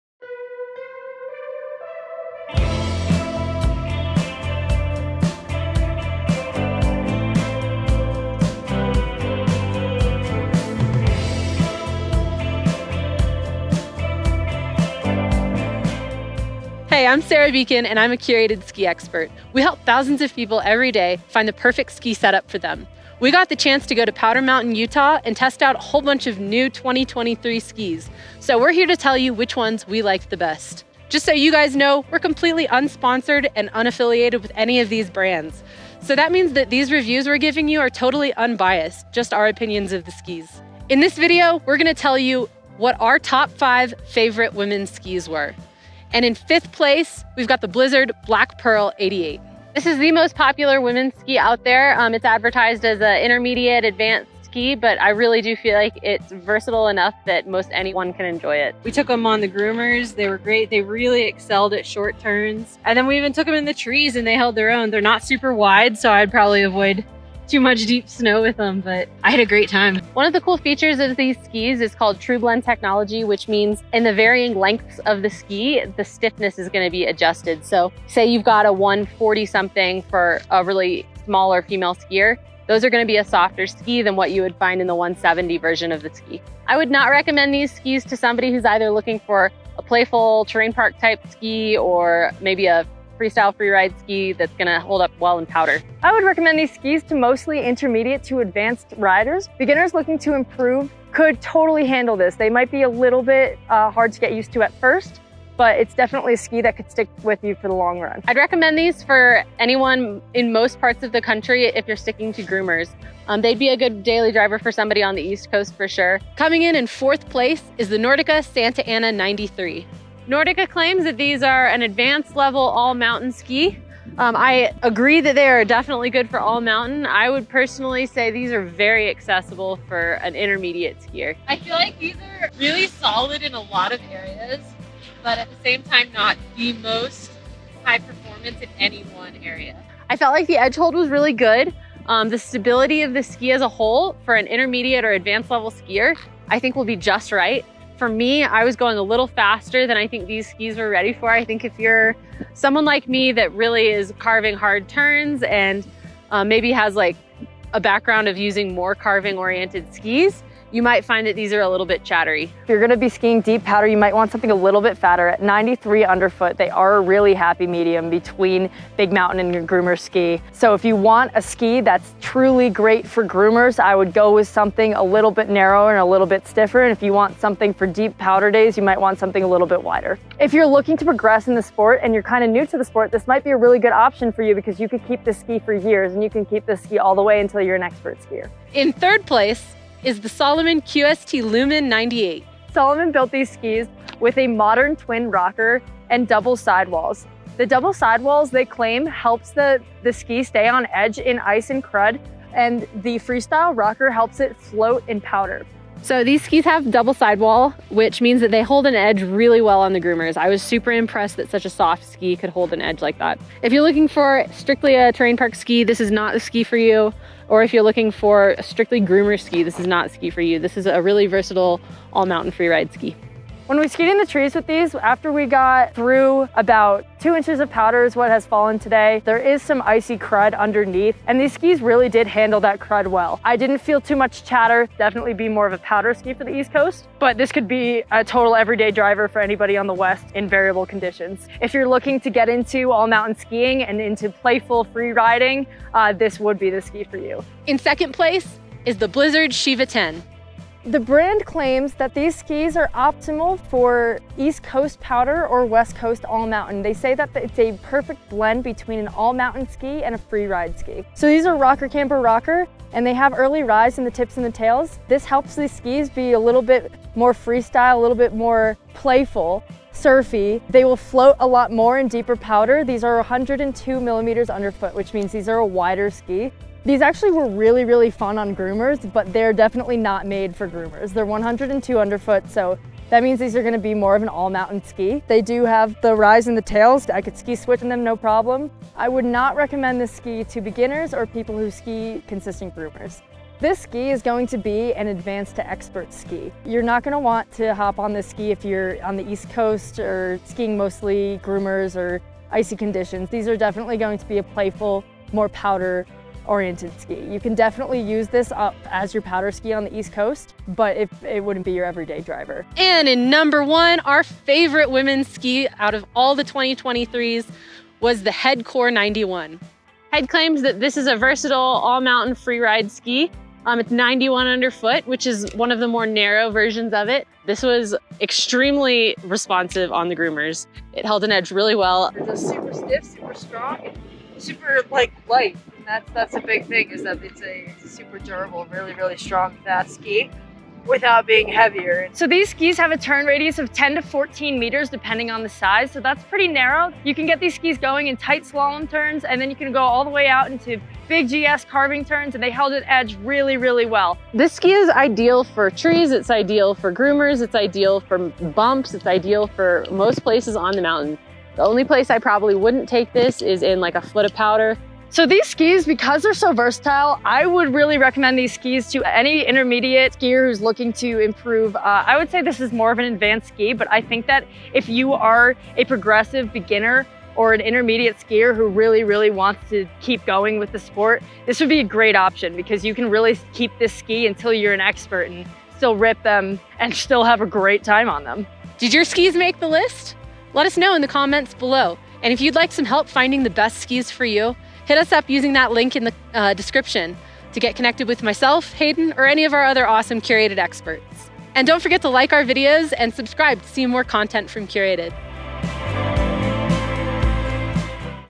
The FIVE 2023 Women’s Skis Curated Experts Love
Curated Experts tested dozens of skis, and these are their top 5 favorite women’s skis for 2023.

0:00 - Curated Experts' Top 5 Women’s Skis
0:46 - Blizzard Black Pearl 88
1:56 - Nordica Santa Ana 93
3:08 - Salomon QST Lumen 98
4:07 - Blizzard Sheeva 10
5:12 - Head Kore 91

If you have any questions about whether these skis are right for you, connect with a Skiing Expert on Curated for free personalized recommendations.